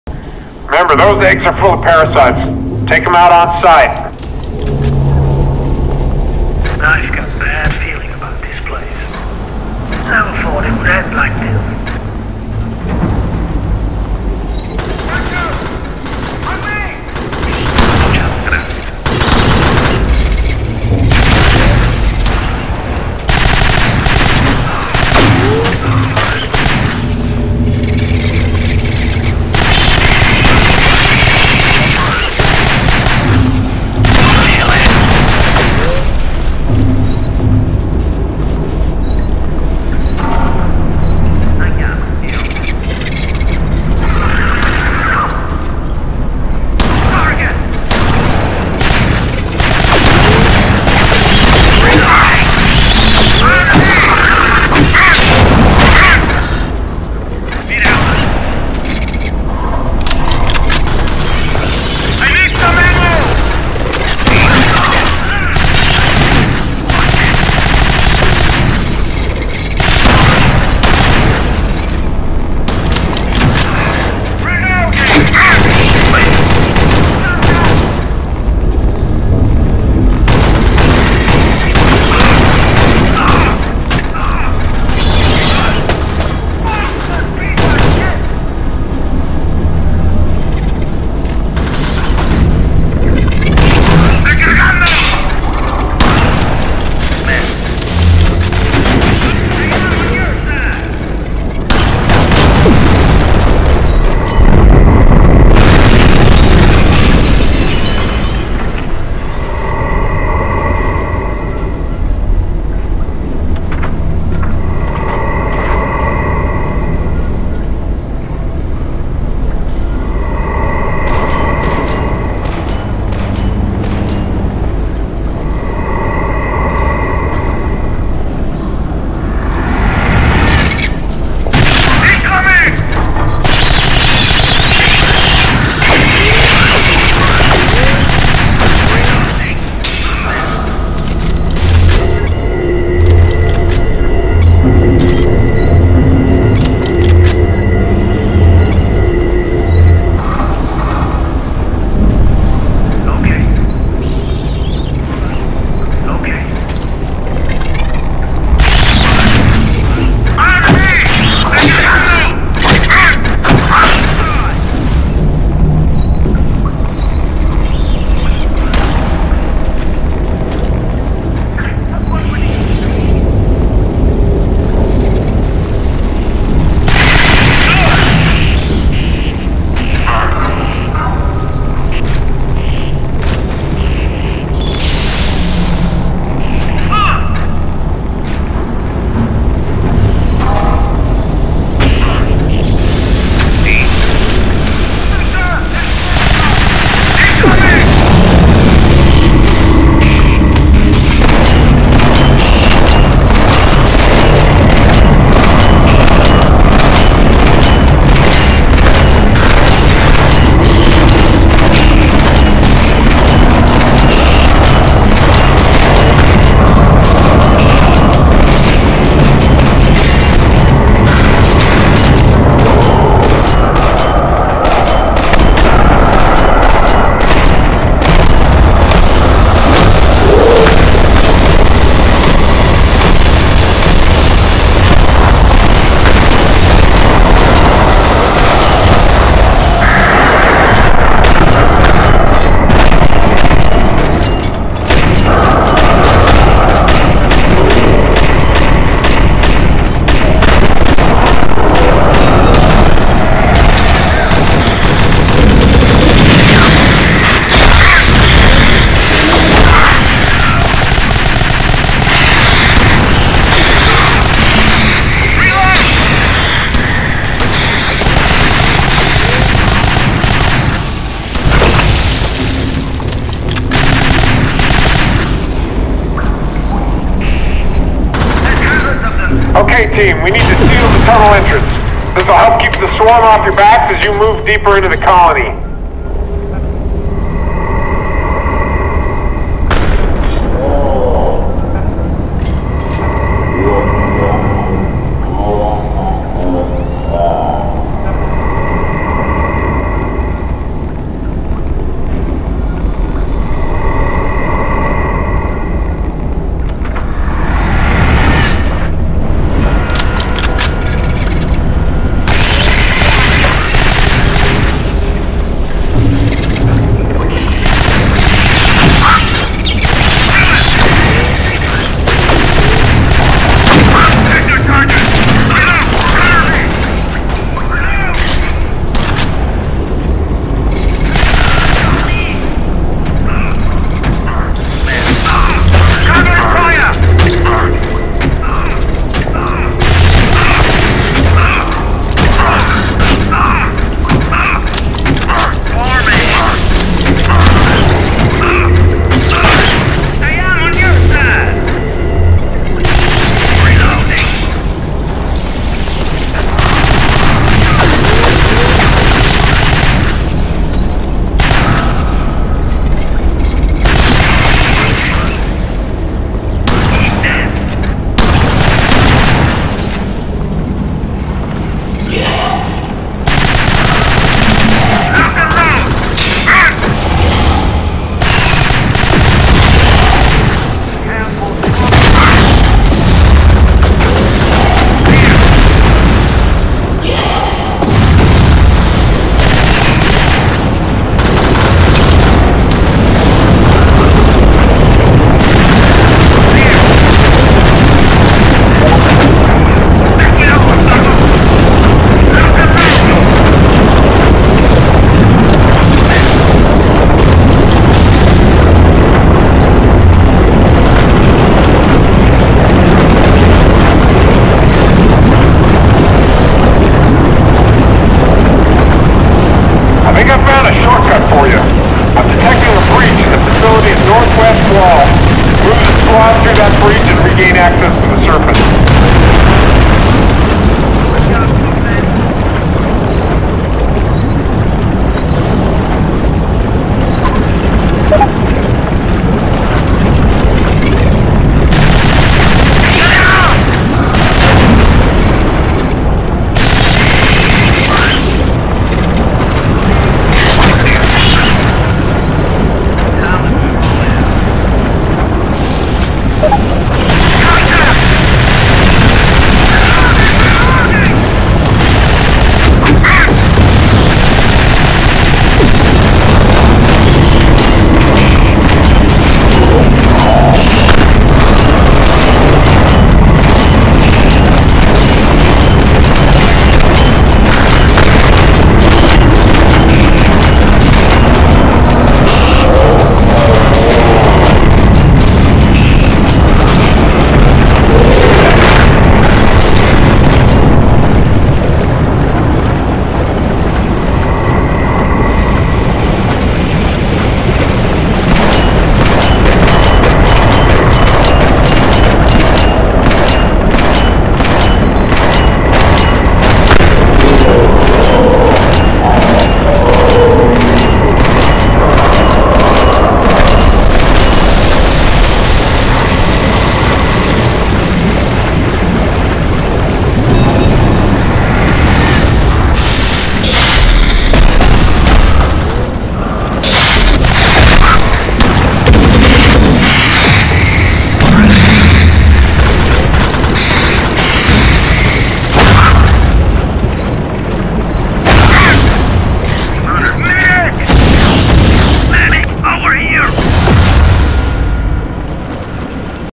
alienz.avi
demos fuck up when you use adrenaline, I guess it can't calculate the movements so it spazzes out. It's not lagging just glitching. It wasn't like that when we were playing though.